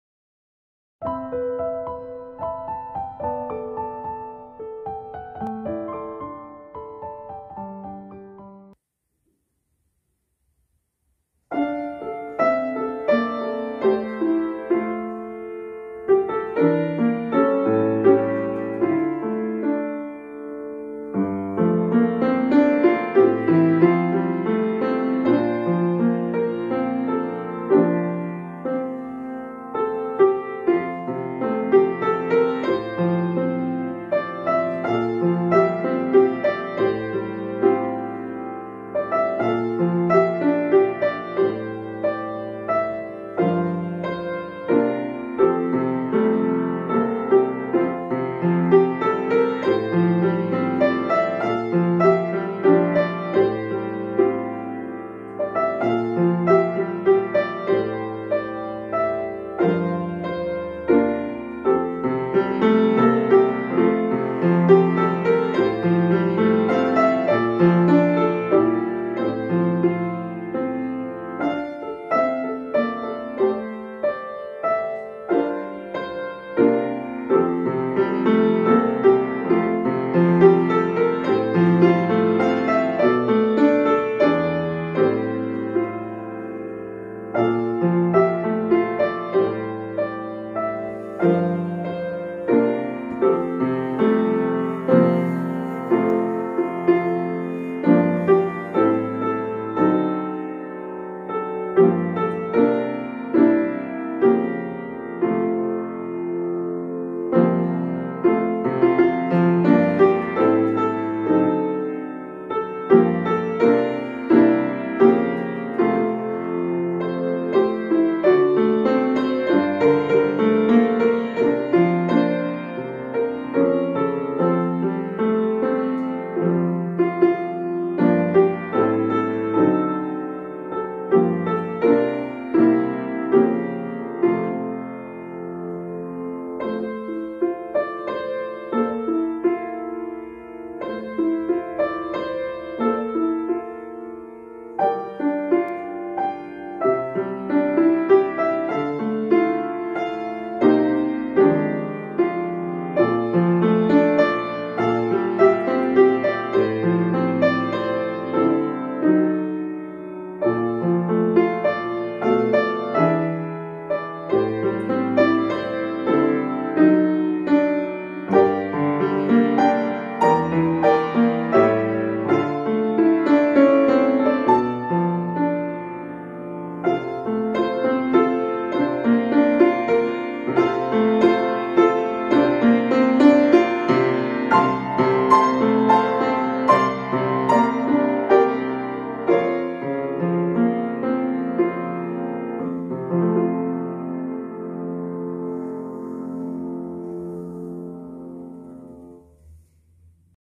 크리스마스 캐롤 메들리 [Christmas Carol Medley Piano Cover.]
다가오는 크리스마스를 위해 캐롤 메들리를 연주해보았습니다.

연주자 : SSO
악보출처 : 마음만은 피아니스트